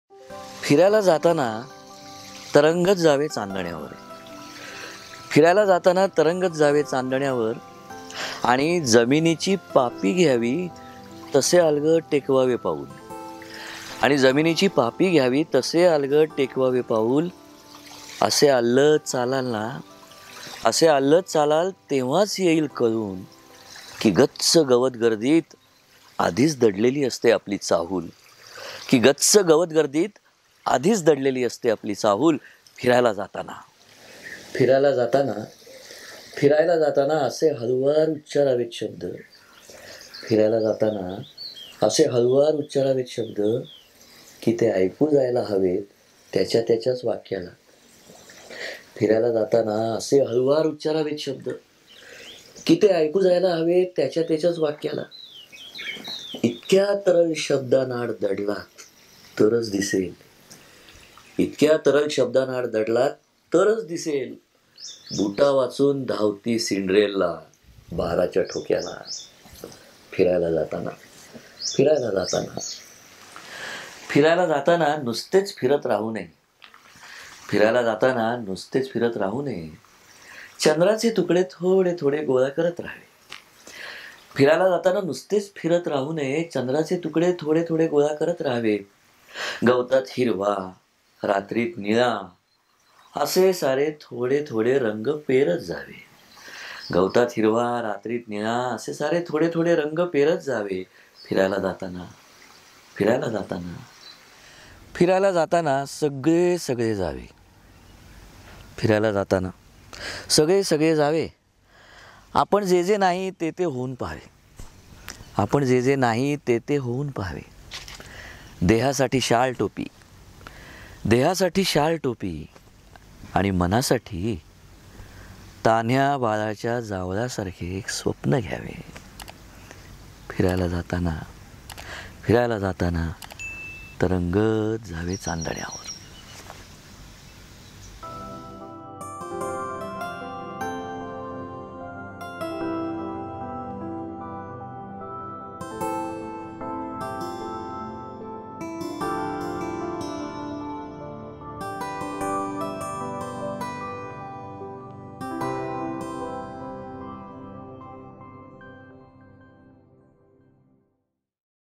कवितेचा कोपरा | Kavitecha Kopara | Sandeep Khare | Marathi Poetry | Marathi Kavita | Episode 33
"कवितेचा कोपरा"
भाग : ३३
कवितेचे शीर्षक : 'फिरायला जाताना...'
प्रकाशन : मौनाची भाषांतरे (कॉन्टिनेन्टल प्रकाशन)
प्रायोजक : जाई काजळ

छायाचित्रण आणि संकलन : सोहम सबनीस, अनिकेत पोंगडे
रंगभूषा : अमृता खानविलकर
लोकेशन पार्टनर : झपूर्झा
डिजिटल पार्टनर : स्मृतिगंध
क्रिएटिव्ह डिझाइन्स : मोनिका साबळे
व्यवस्थापन : कुशल खोत, सौमित्र कुलकर्णी
सहयोग : वाइड विंग्ज मीडिया
कविता आणि सादरीकरण : संदीप खरे